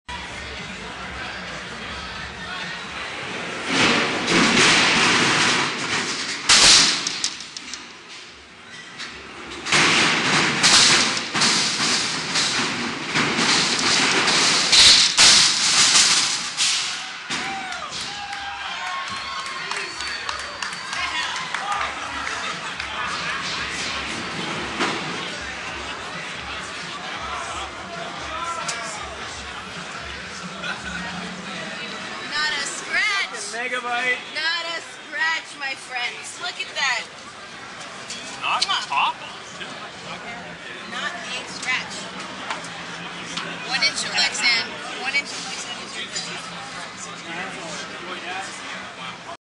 Sony GDC: Ziggy Vs SuperMegabyte
Superheavy weight monsters get it on at Combots SonyGame Developers conference event.